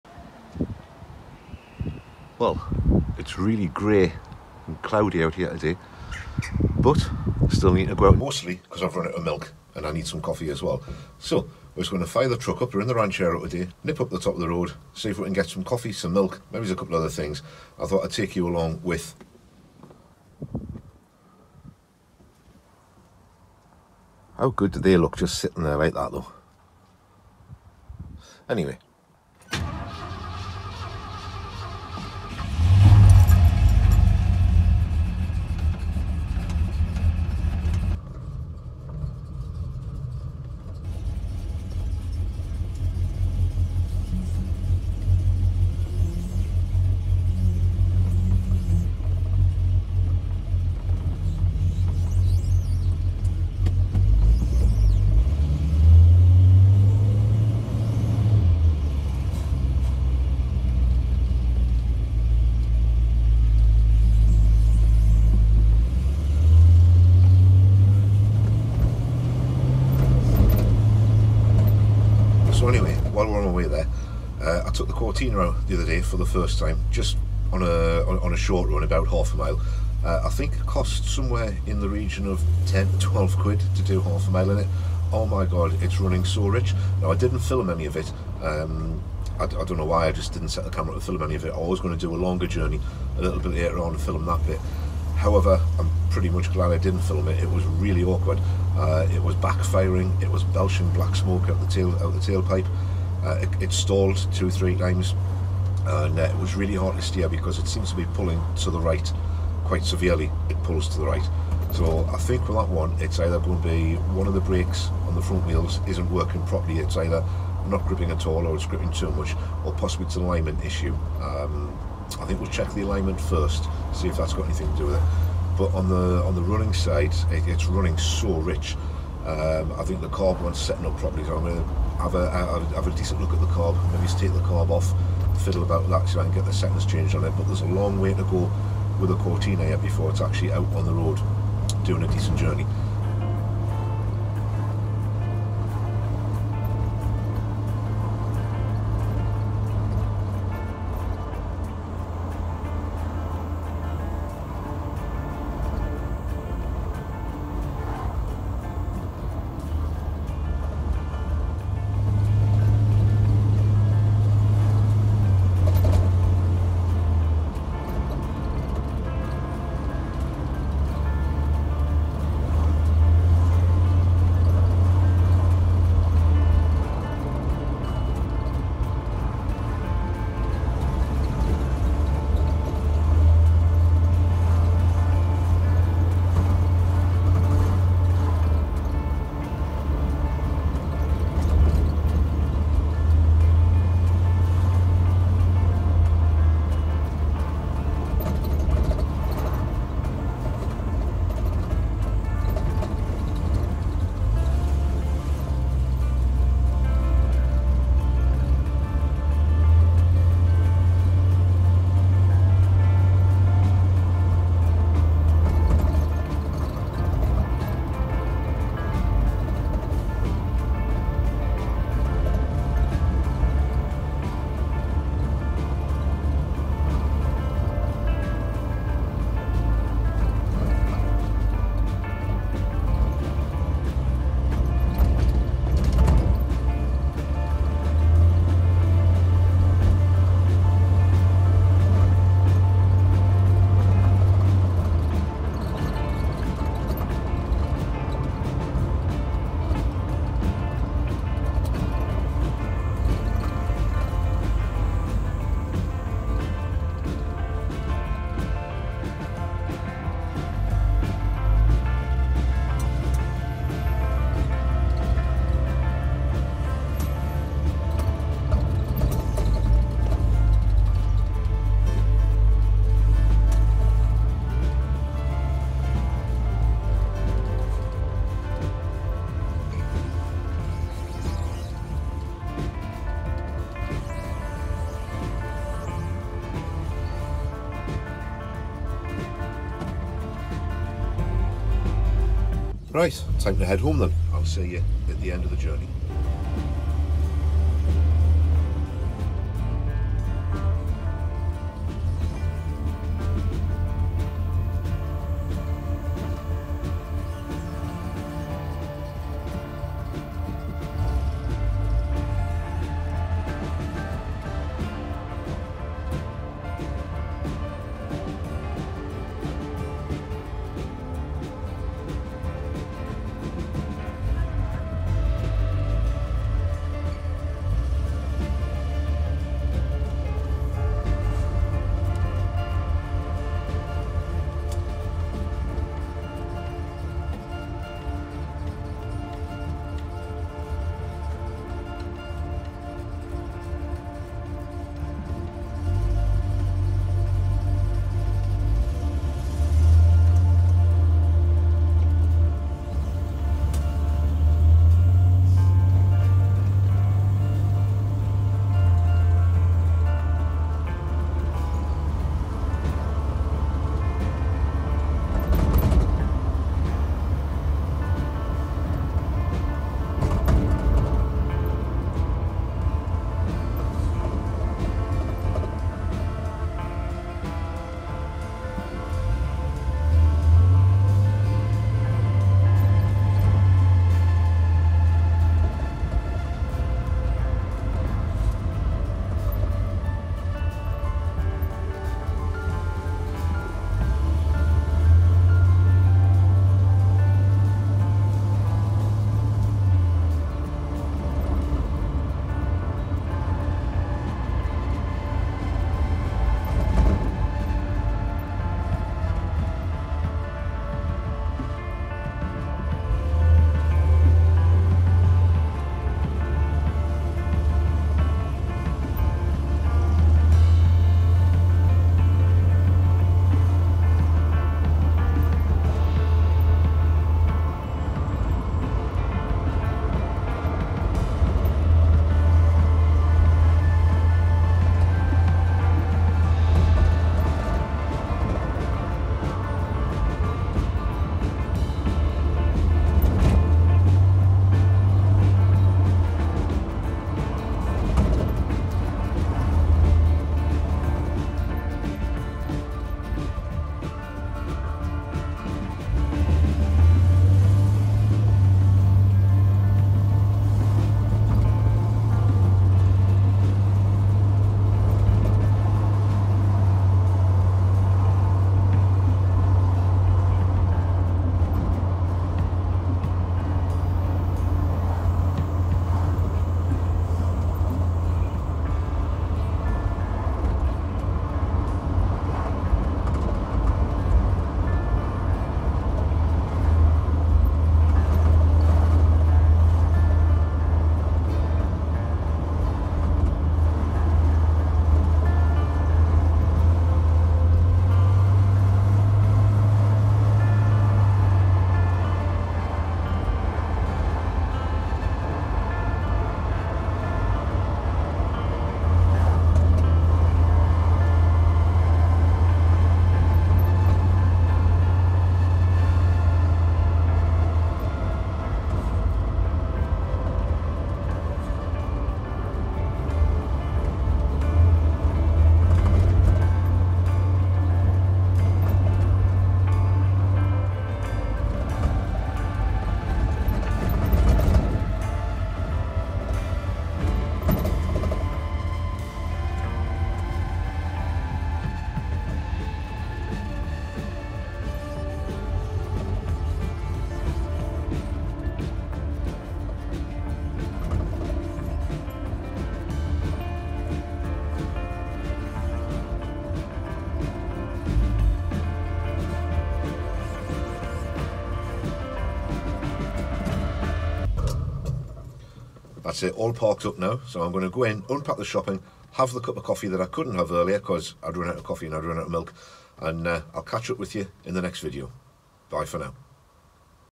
Daily Driving The Ranchero
Taking the Ranchero for a drive to get some groceries. Even on the greyest of grey days, it's good to go for a drive, especially when you're in something you enjoy owning and driving.